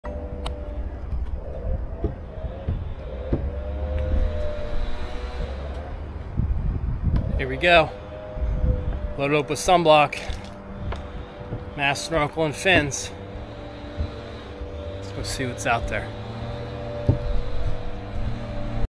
DJI Action 4 and the Gopro 12 before I take them in the ocean!
I unbox the Action 4 from DJI and the Gopro 12 in my studio in Boca Raton before I take them into the water!